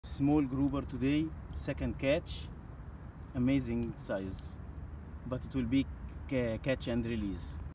1st Video of 2nd Strike Grouper @ Abu Dhabi, Shore Casting technique on 07/03/2023
1st Video of 2nd Strike Grouper @ Abu Dhabi, Shore Casting technique on 07/03/2023

 صيد سمكه الهامور بأبوظبي علي اللور الريد هيد

ROD  Daiwa EXCELER EXC802MFS 8' 1/8-3/4oz 6-14lb
REEL  Daiwa LAGUNA LT3000-C
Braid  VARIVAS Avani Cashing PE X8 20.2lb 1PE
Leader Varivas Shock Leader 16lb 8.0kg 0.34mm
Snaps  AFW #2 25lb 11kg
Lure Duel Heavy Sinking Minnow 90s 26g